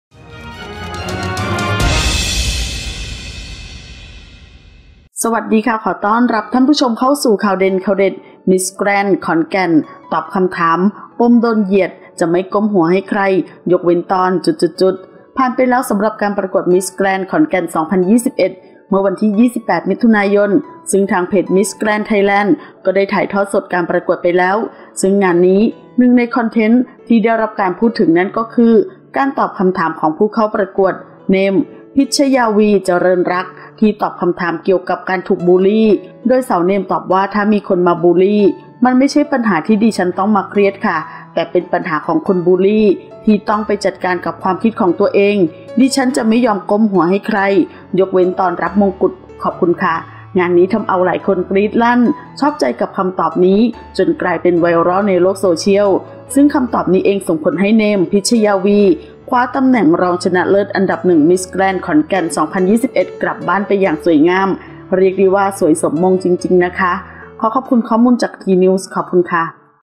มิสแกรนด์ขอนแก่น ตอบคำถาม ปมโดนเหยียด จะไม่ยอมก้มหัวให้ใคร ยกเว้นตอน...
ขอบคุณที่มา :
ขอขอบคุณข้อมูลจาก: